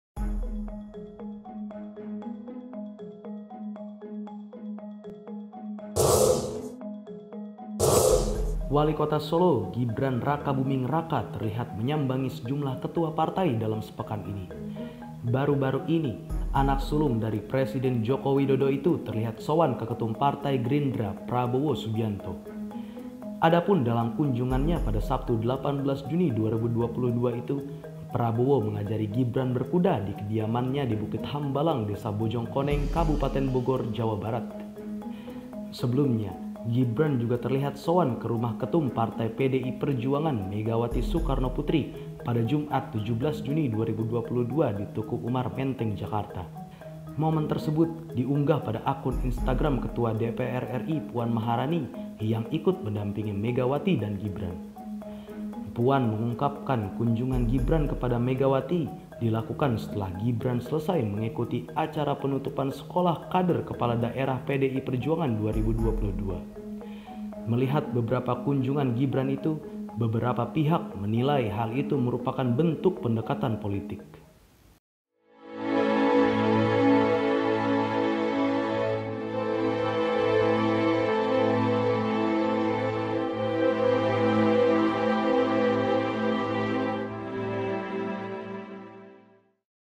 Berkuda Bersama Prabowo, Gibran Sowan ke Sejumlah Ketum Partai
Wali Kota Solo Gibran Rakabuming Raka terlihat menyambangi sejumlah ketua partai dalam sepekan ini.

Baru-baru ini, anak sulung dari Presiden Joko Widodo itu terlihat sowan ke Ketum Partai Gerindra Prabowo Subianto.

Adapun dalam kunjungannya, pada Sabtu 18 Juni 2022 itu, Prabowo mengajari Gibran berkuda di kediamannya, di Bukit Hambalang, Desa Bojong Koneng.

Sebelumnya, Gibran juga terlihat sowan ke rumah ketum partai PDIP Megawati Soekarnoputri, pada Jumat 17 Juni 2022, di Teuku Umar Menteng Jakarta.

Momen tersebut diunggah pada akun Instagram Ketua DPR RI Puan Maharani, yang ikut mendampingi Megawati dan Gibran.

Puan, mengungkapkan kunjungan Gibran kepada Megawati dilakukan setelah Gibran selesai mengikuti acara penutupan Sekolah Kader Kepala Daerah PDI Perjuangan 2022.

Melihat beberapa kunjungan Gibran itu, beberapa pihak menilai hal itu merupakan bentuk pendekatan politik.